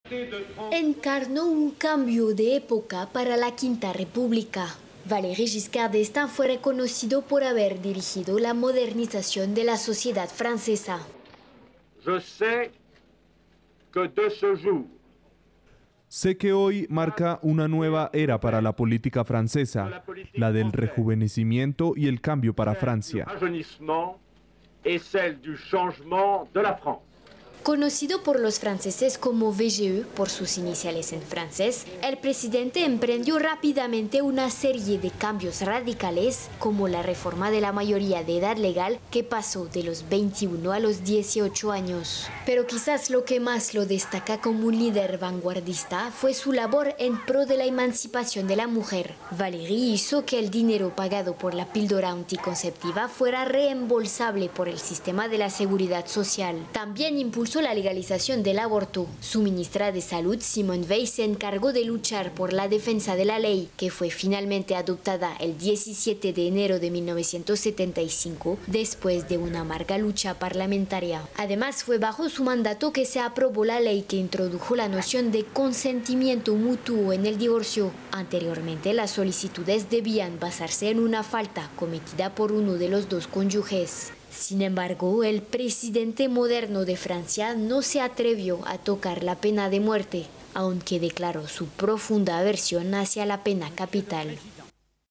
Valéry Giscard d'Estaing, un político francés vanguardista
Murió a los 94 años el expresidente Valéry Giscard d'Estaing, quien lideró a Francia entre 1974 y 1981. Así lo confirmó el Elíseo y fuentes cercanas al exmandatario, el más joven en llegar al poder en ese país en su momento, hasta que Emmanuel Macron fue elegido con 39 años.